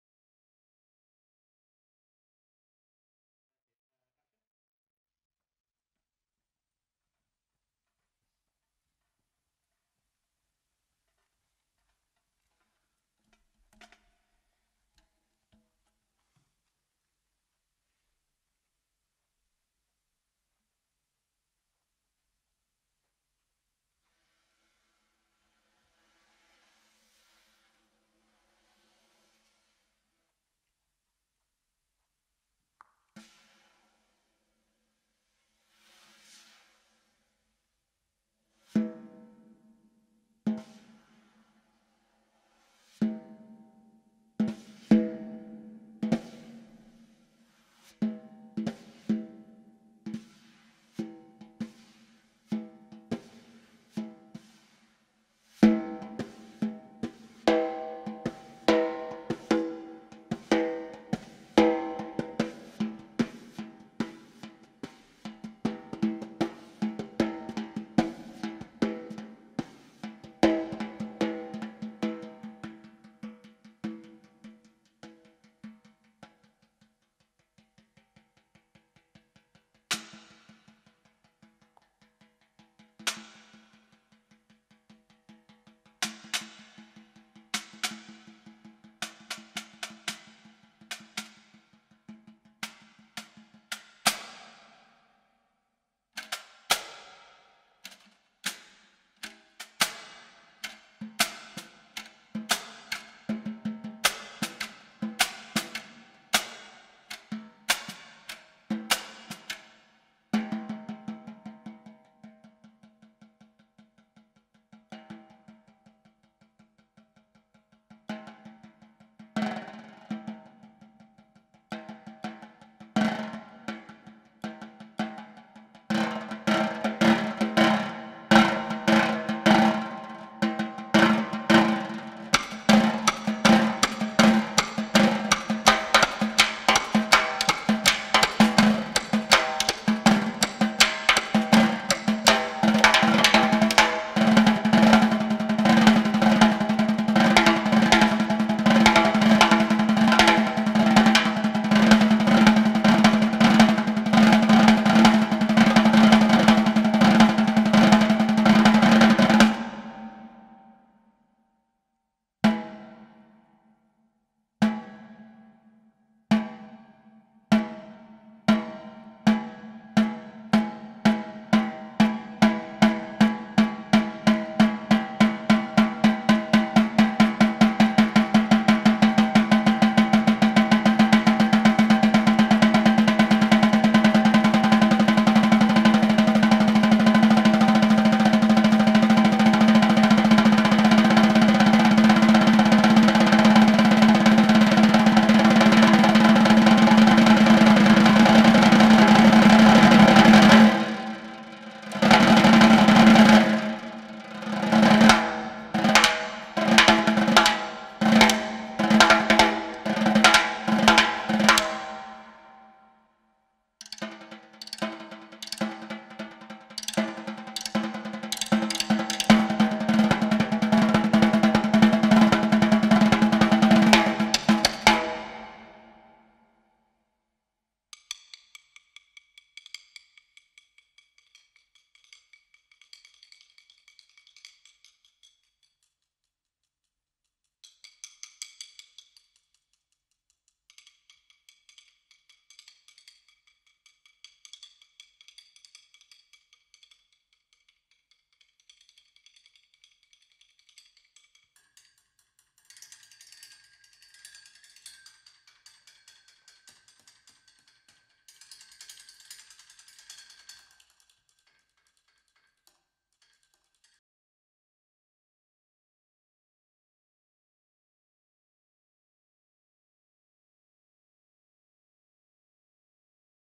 Barbican Sessions: Christoph Sietzen
For our latest Barbican Session, percussionist Christoph Sietzen performs in an empty Cinema 1.

Each month, we invite artists to perform a live session in our spaces, from our Conservatory to our highwalks and beyond

Mixing and mastering: Michele Gaggia / Digital Natural Sound